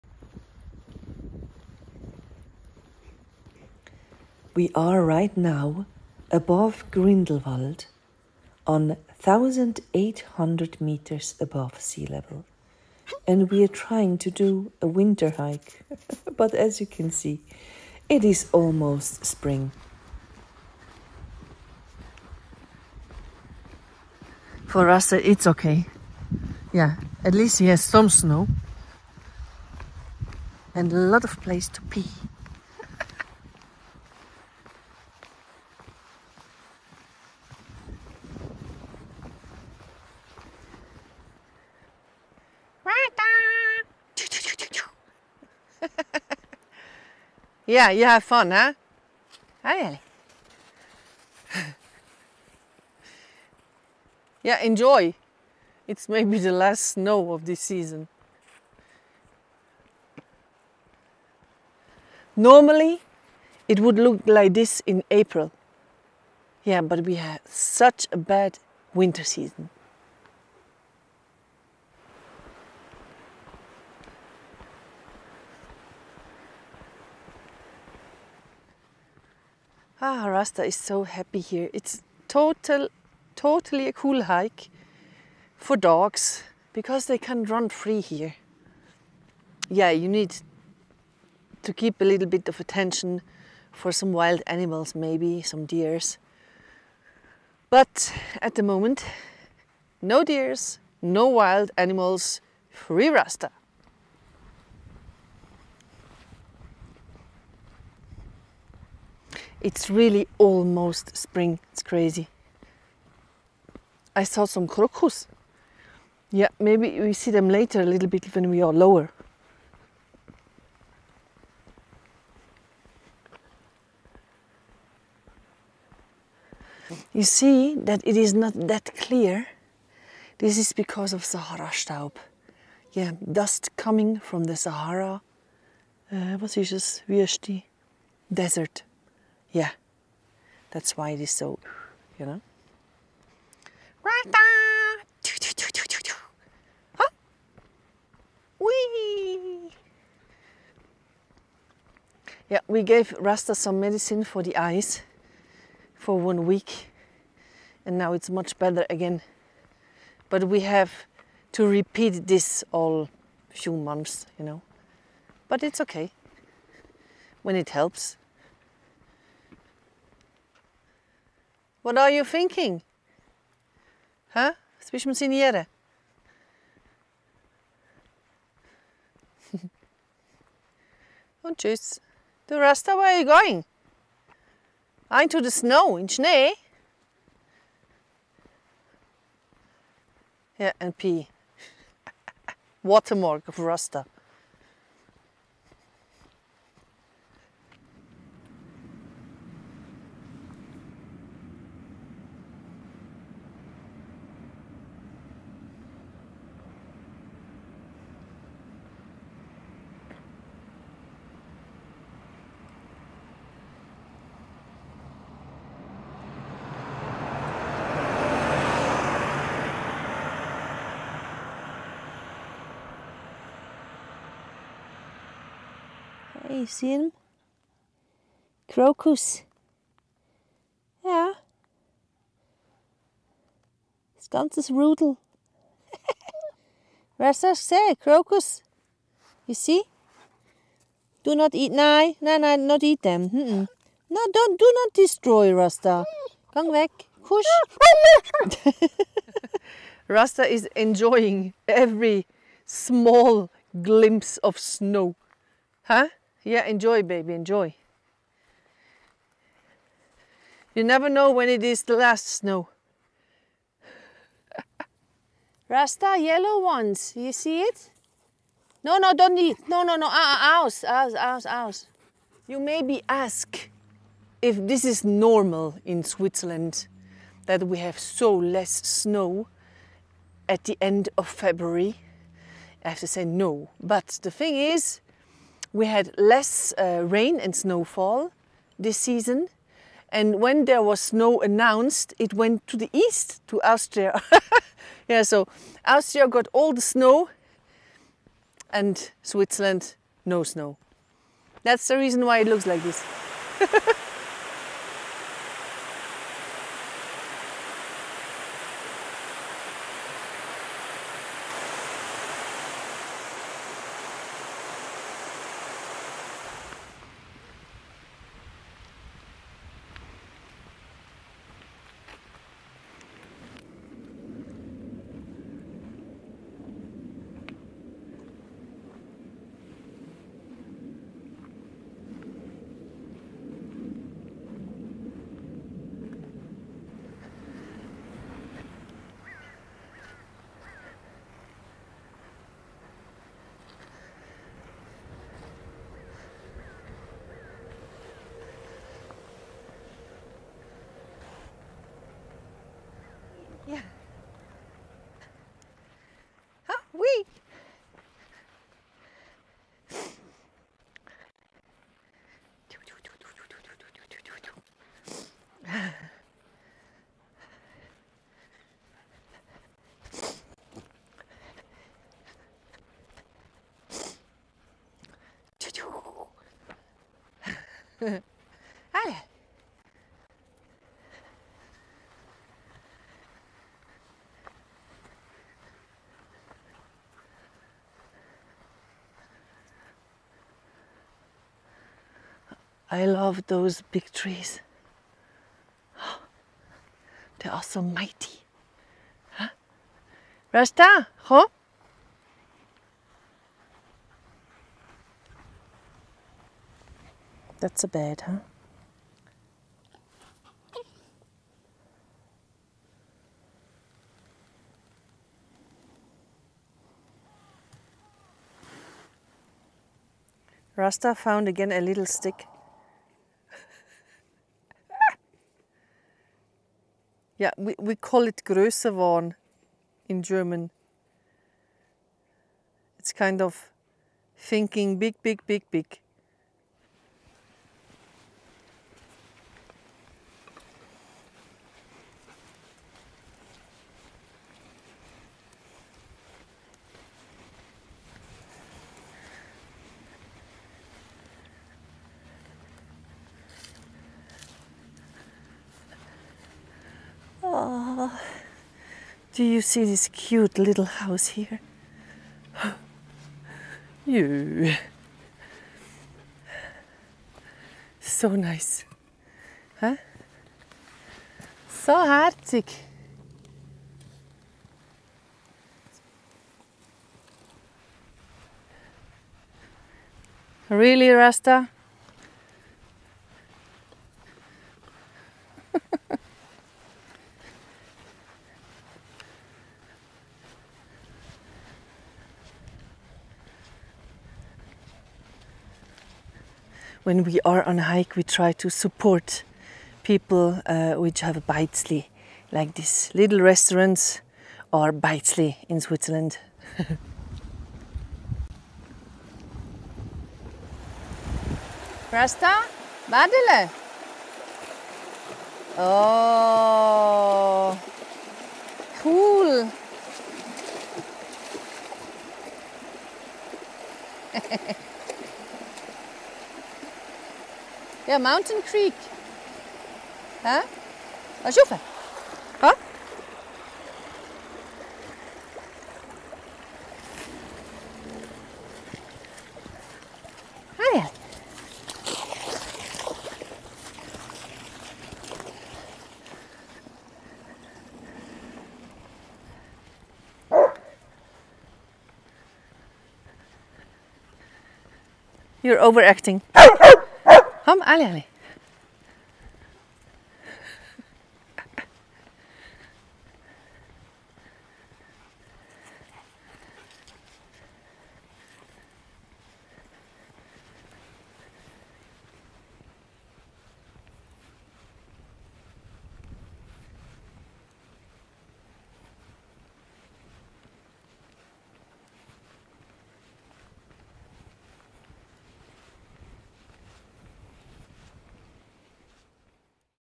Hiking with dogs in Swittzerland 4K
0:00 Starting our spring-winter hike at Bussalps Grindelwald
4:02 Rasta is trying to eat Krokus but they are not healthy for dogs!
4:50 Explanation why there is no snow here in February
5:52 View point
6:50 Almost sleeping in the moss
8:57 Having fun in the creek

Video @SylviaMichelPhotography

The life of Rasta - the white shepherd

It is a really snowless winter in Switzerland. But apparently we will have to get used to it. My mom and I still had a lot of fun on our hike. For me, there was still enough snow to bite into and pee in. So the world is fine for me :)